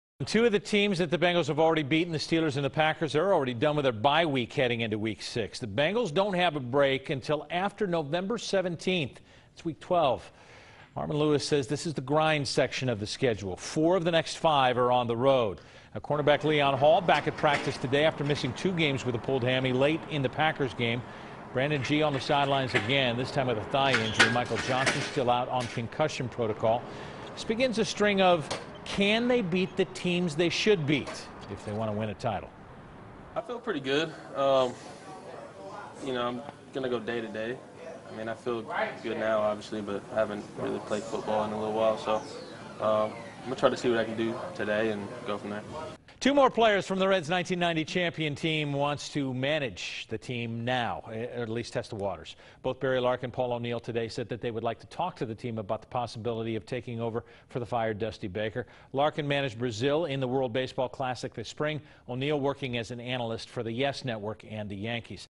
Daily Sportscast: Wednesday, October 9
CINCINNATI (WKRC) -- Brad Johansen has Bengals and Reds news